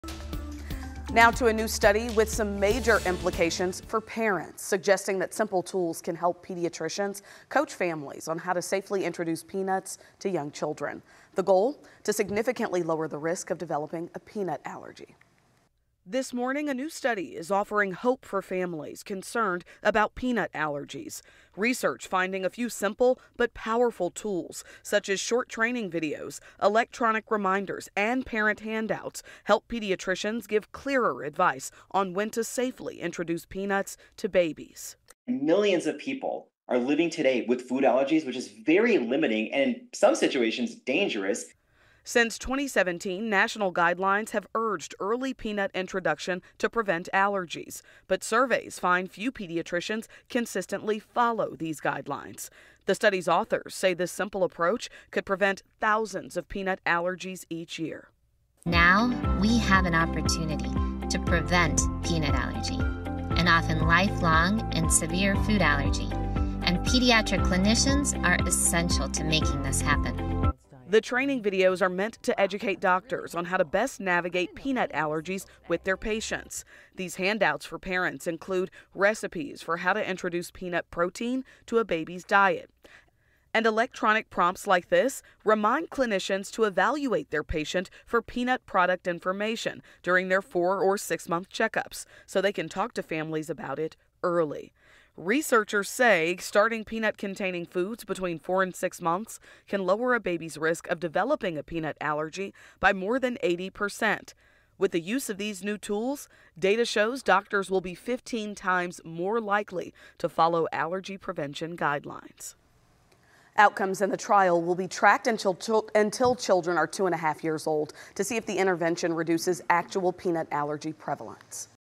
New study suggests safer ways to introduce peanuts to children
Simple tools helped give doctors clearer advice on introducing peanuts to babies.